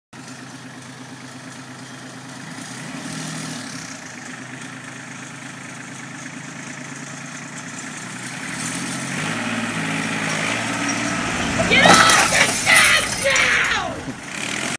Tractor Accident
My dad has a little mishap with his tractor.